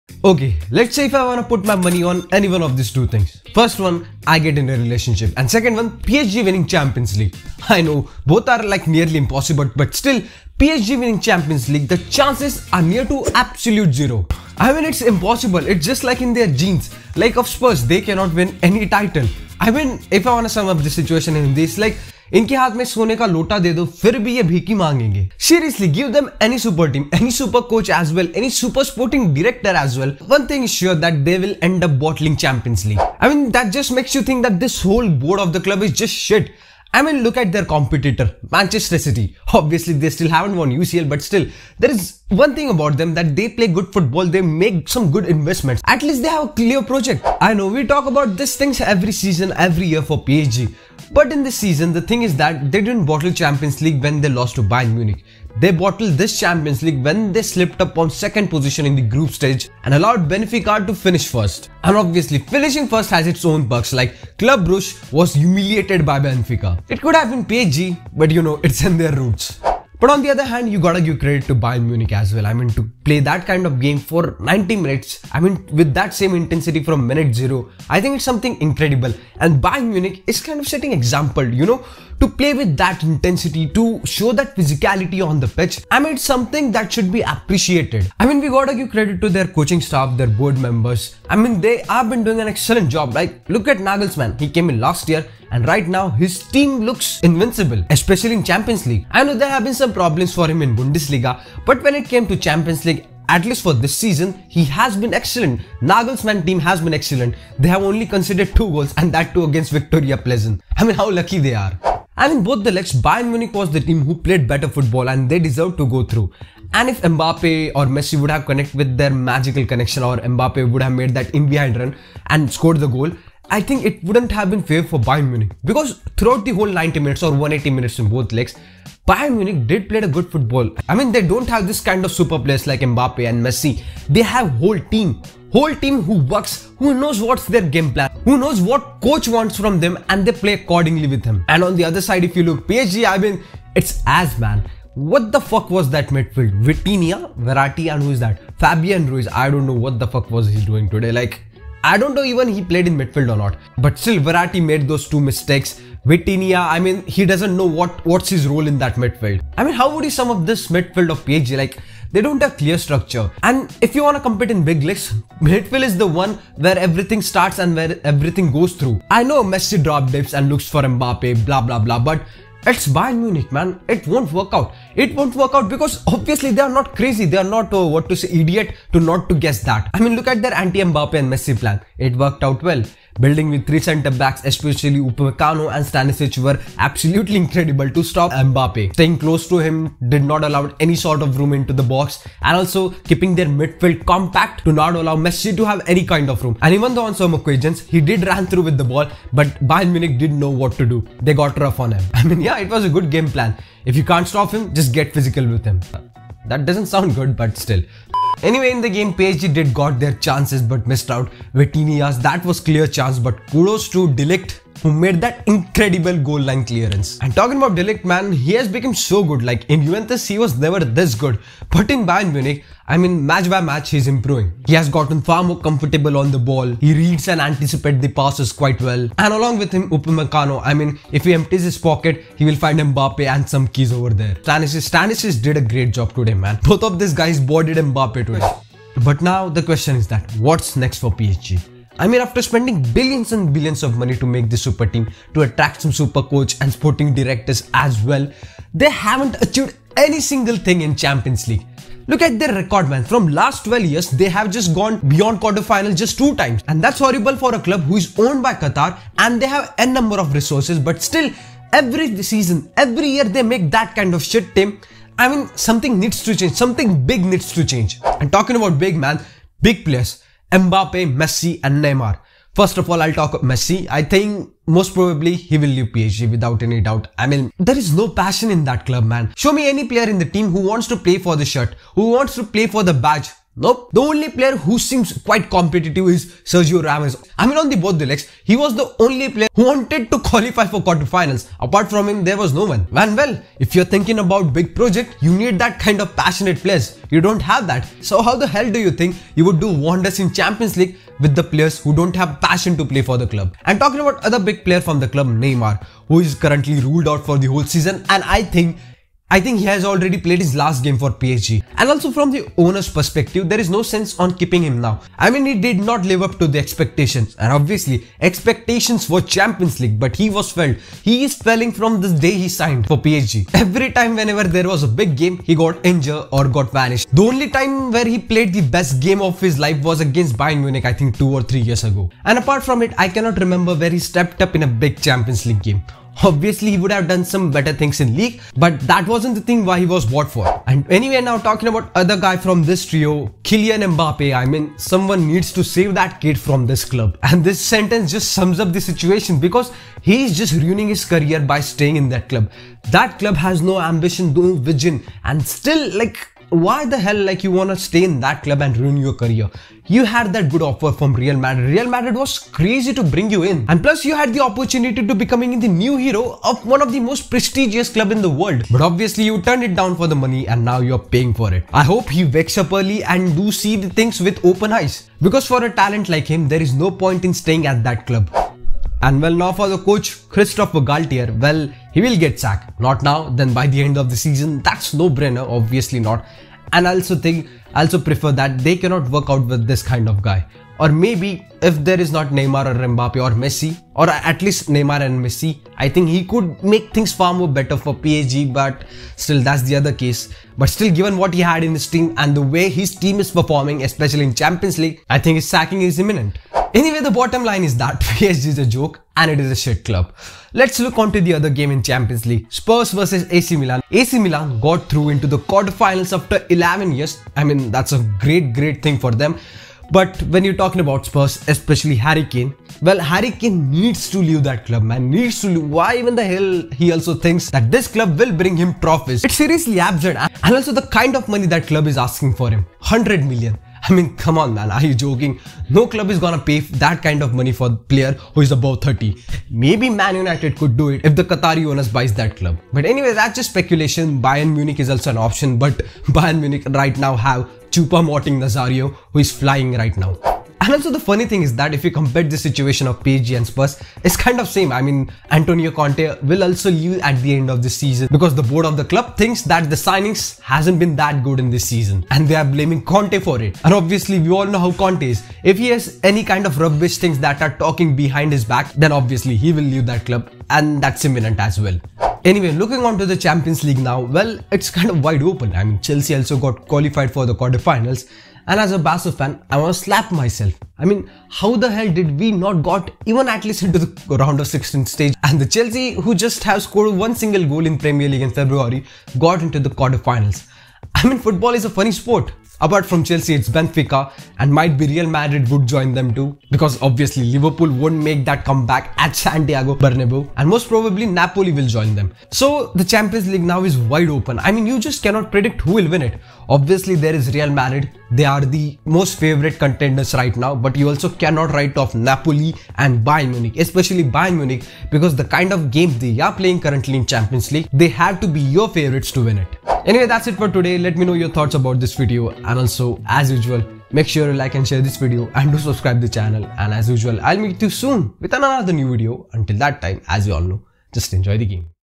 PSG IS AN ABSOLUTE JOKE! | Messi and Neymar to Leave PSG?
Bayern Munich 2-0 PSG. Bayern knocks out Psg from champions league.